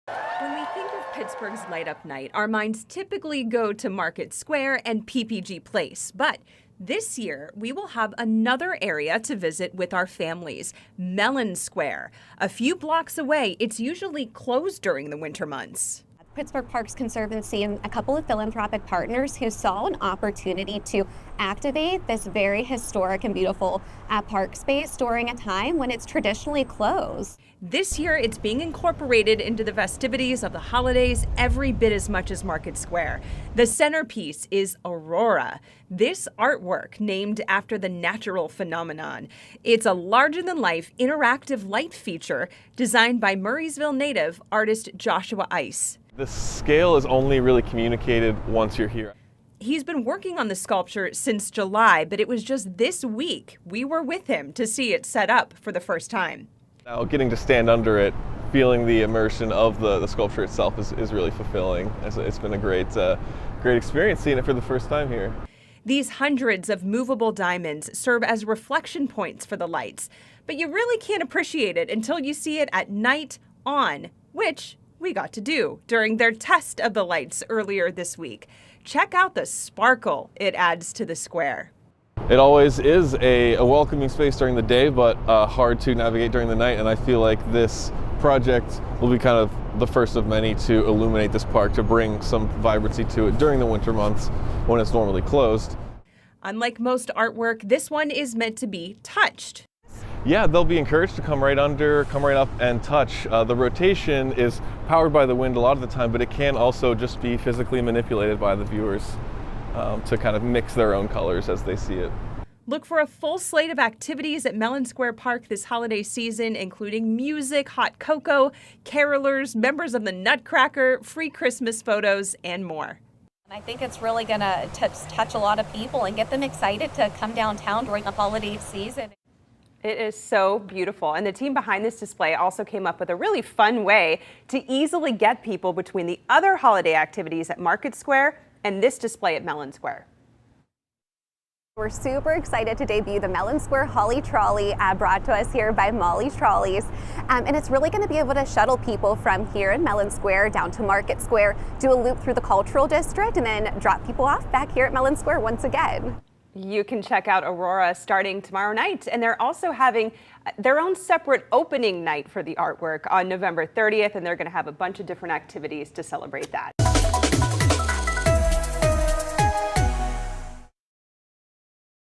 Mellon Square joins Pittsburgh's Light Up Night; debuts Aurora light sculpture
Mellon Square Park will join Pittsburgh's Light Up Night festivities, adding a new Downtown destination for families to enjoy. The centerpiece of the holiday display is "Aurora," an interactive light sculpture designed by artist Joshua Ice, a Murrysville native.